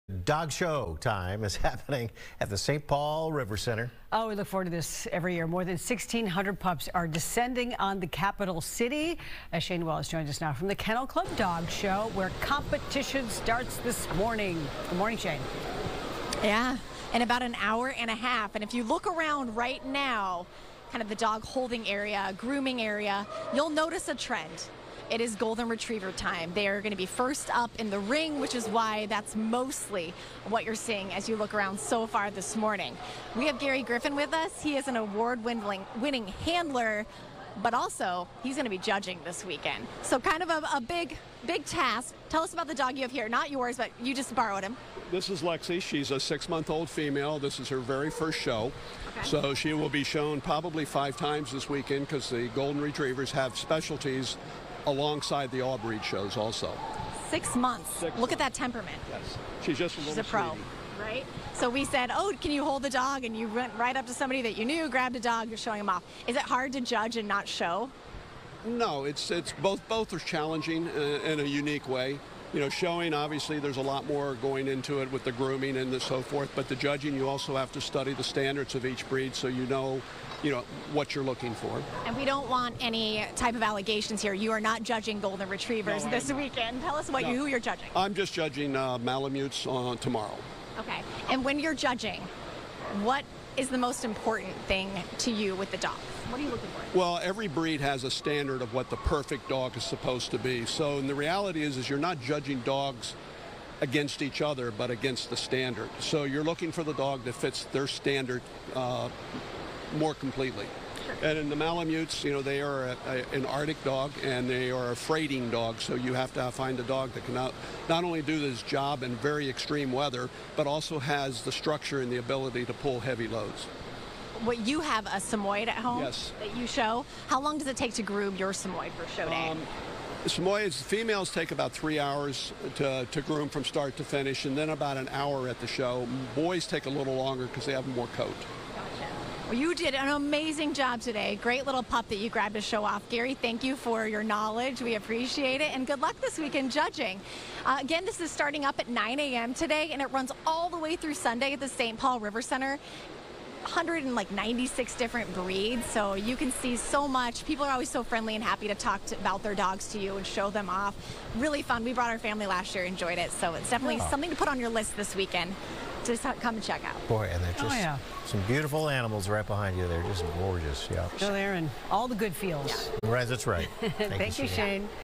Land O’ Lakes Kennel Club Dog show
The St. Paul RiverCentre is hosting more than 1,600 pups for the annual Land O’ Lakes Kennel Club Dog show.